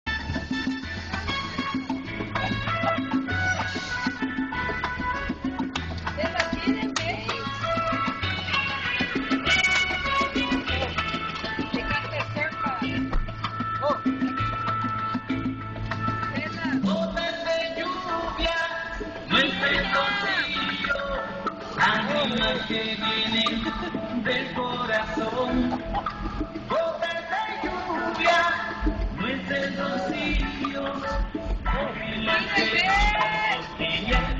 Albert making a crowd @ foodtown
excuse my grandparent they were trying to get him in the car lol he just loves to dance